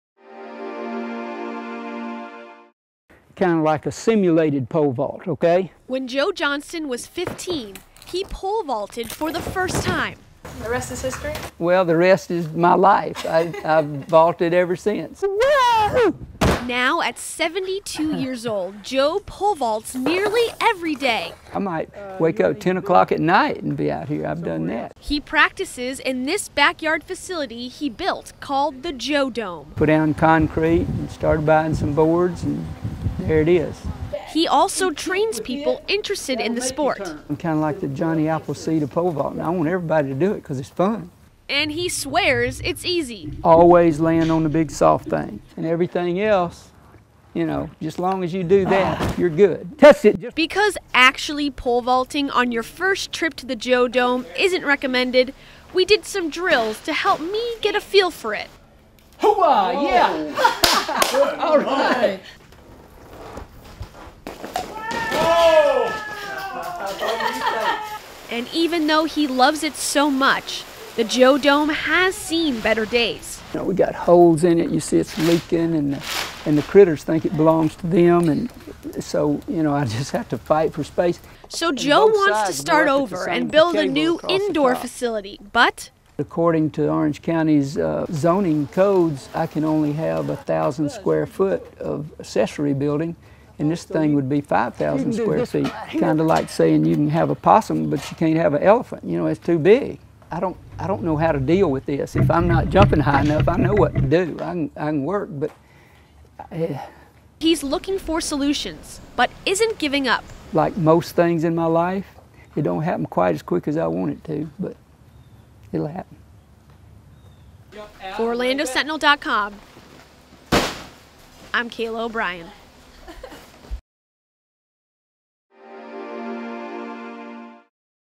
72 Year Old Pole Vaulter Trying to Build Backyard Training Facility
72-year-old Joe Johnston is trying to build an indoor pole-vaulting facility in his backyard to replace the outdated Joe Dome.